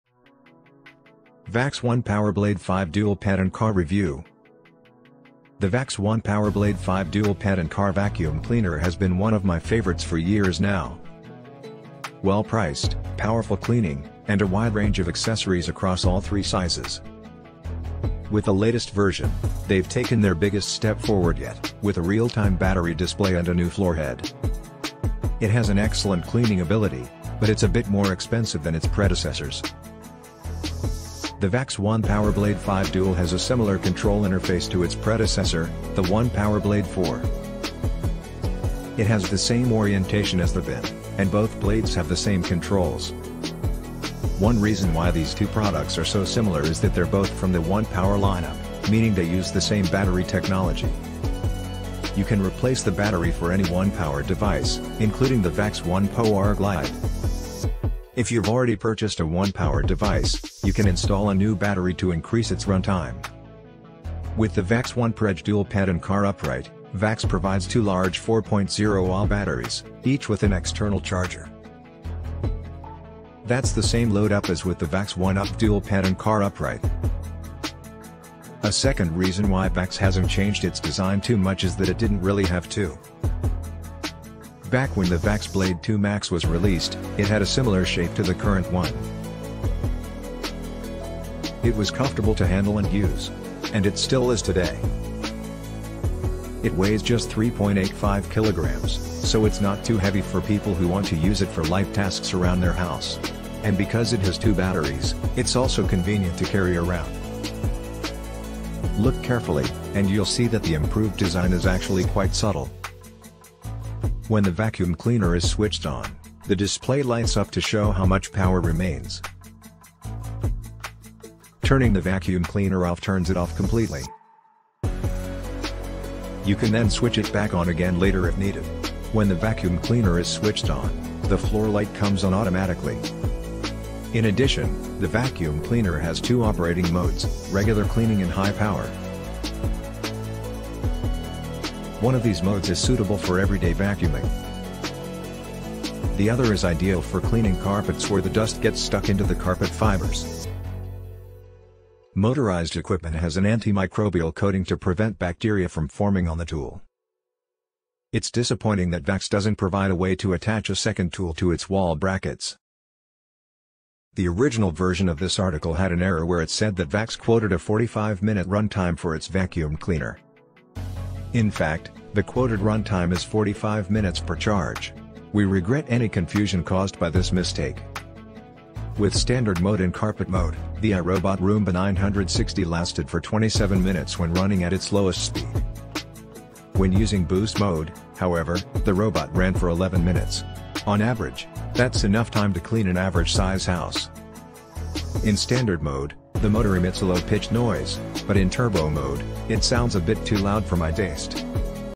Vax ONEPWR Blade 5 Dual Pet & Car Review | Lots of Accessories
Amazon Product Name : Vax ONEPWR Blade 5 Dual Pet & Car Review

Looking for a powerful and versatile cordless vacuum cleaner for your home and car? Check out the Vax ONEPWR Blade 5 Dual Pet & Car! In this video, we'll take a closer look at this high-performance cleaning machine, examining its features, performance, and overall value. Whether you're a pet owner, busy parent, or simply looking for a way to keep your home and car clean and tidy, the Vax ONEPWR Blade 5 has got you covered. So sit back, relax, and join us as we explore the world of cordless vacuum cleaners with the Vax ONEPWR Blade 5 Dual Pet & Car review.